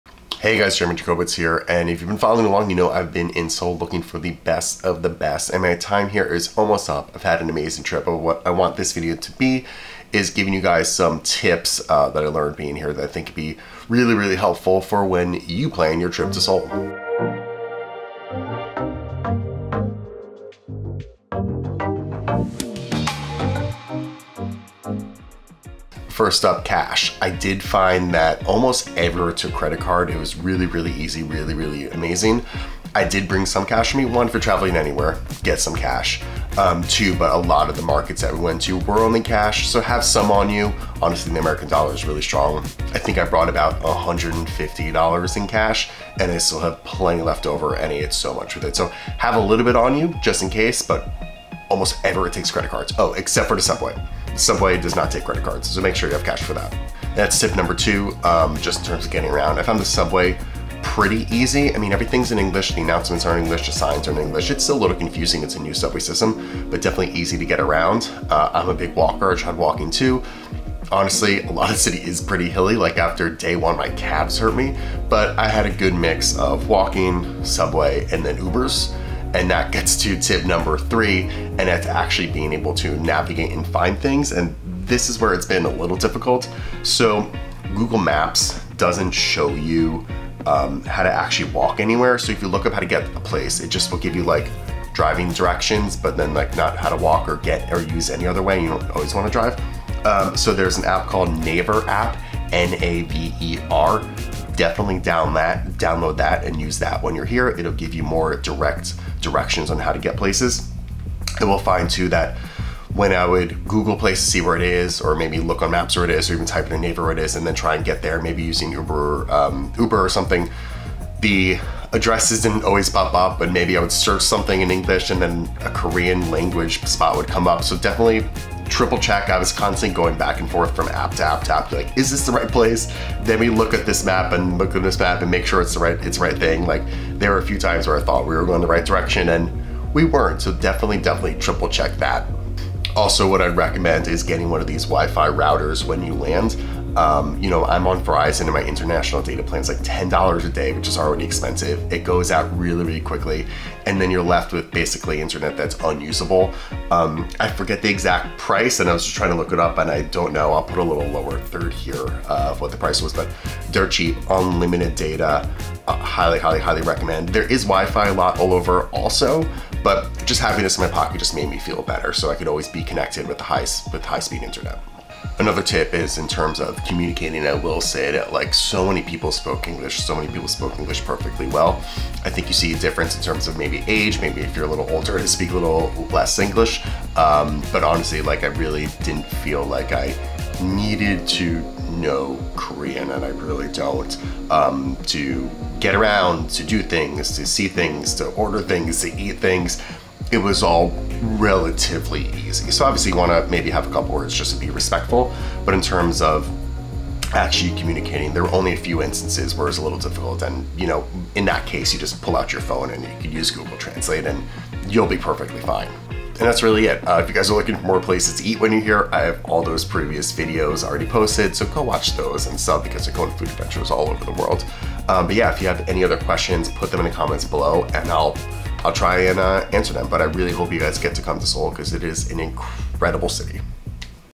Seoul for First-Timers! 5 MUST TIPS to KNOW | Jeremy Jacobowitz
Jeremy Jacobowitz is in Seoul, South Korea on the hunt for the best food in the city! Today he is giving you 5 travel tips that helped him tremendously!